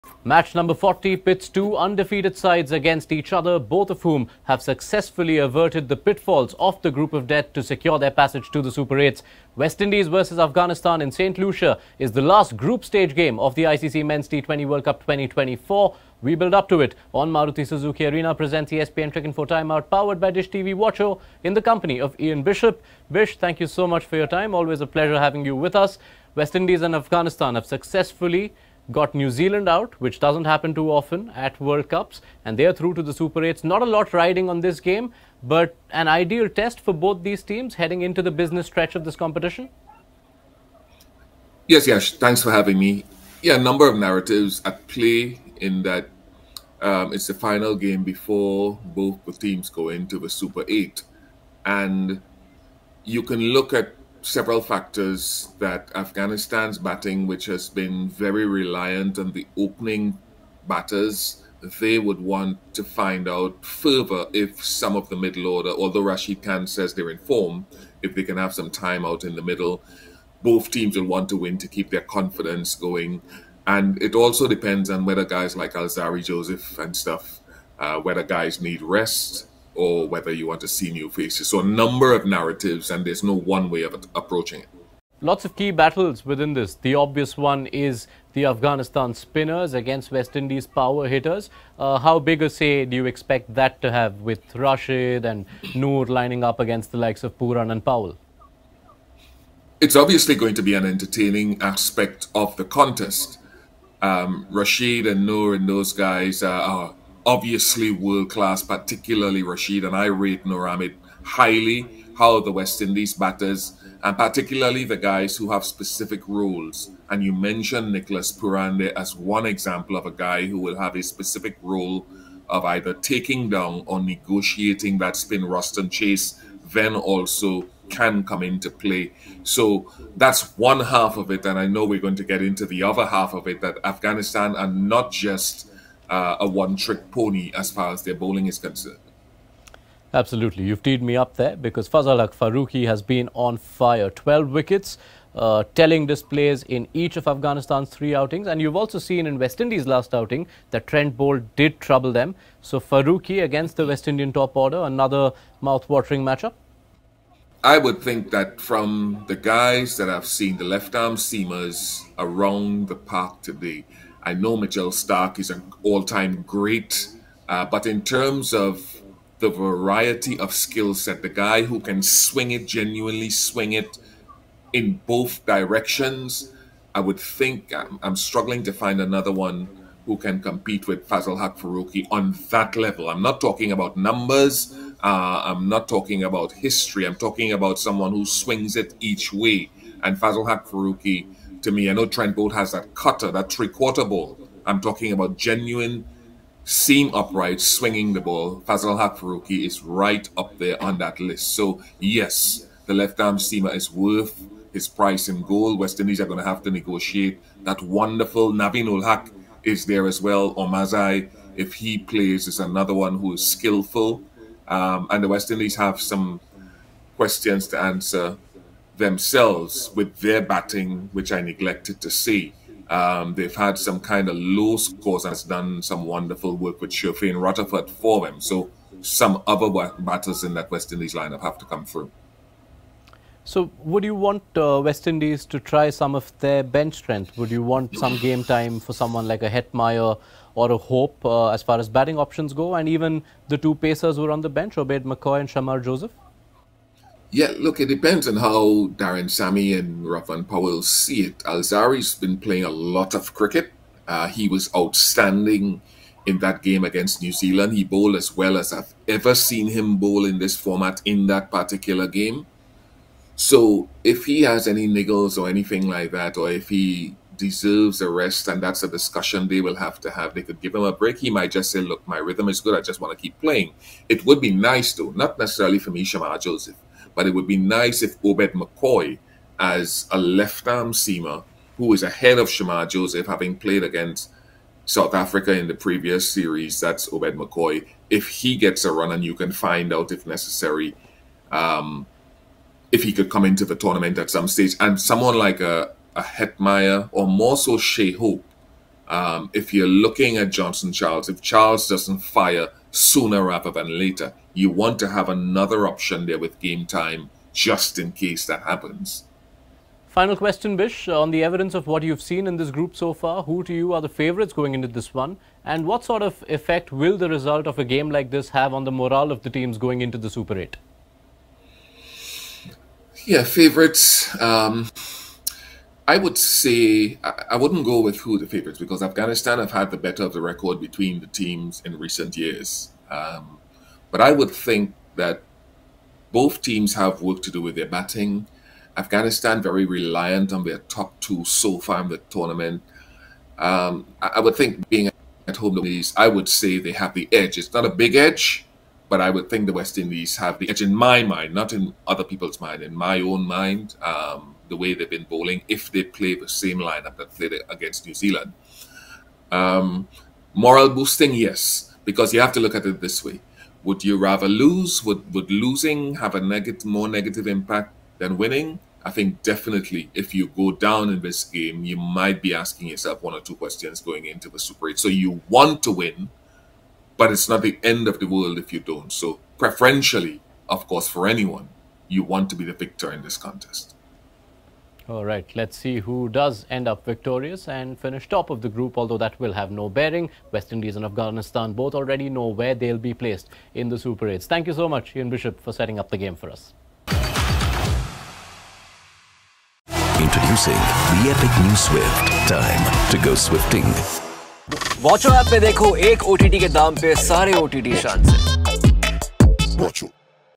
WIvsAFG Match Preview | T20 World Cup 2024 - West Indies vs Afghanistan
What will be the key battle? AFG spin vs WI power hitters? Or Farooqi's guile against the WI top order? Ian Bishop previews the clash in Gros Islet.